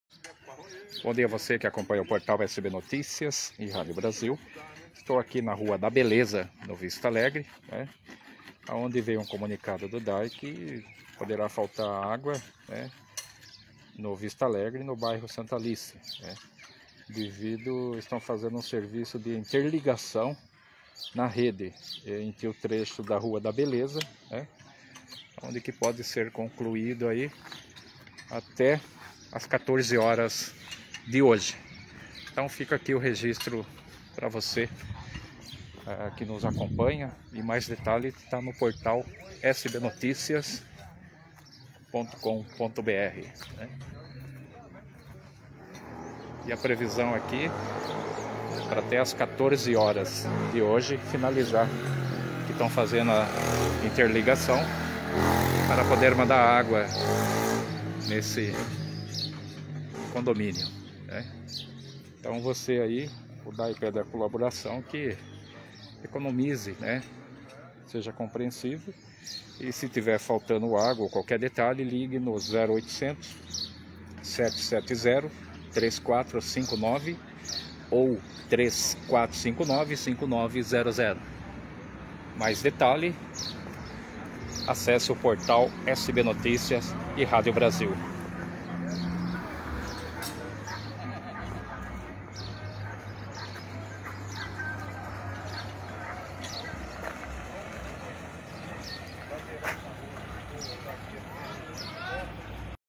Interligação de rede pode afetar abastecimento de água no Vista Alegre e Santa Alice
O Departamento de Água e Esgoto (DAE) de Santa Bárbara d’Oeste informa que, nesta quinta-feira (18), será realizada a interligação de rede na região do Vista Alegre. Em função do serviço, o abastecimento poderá ser afetado.

O serviço esta sendo realizado na Rua da Beleza, a autarquia informou que o fornecimento de água do bairro Santa Aline também será interrompido. A previsão de conclusão e normalização no abastecimento deve ocorrer por volta das 14 horas.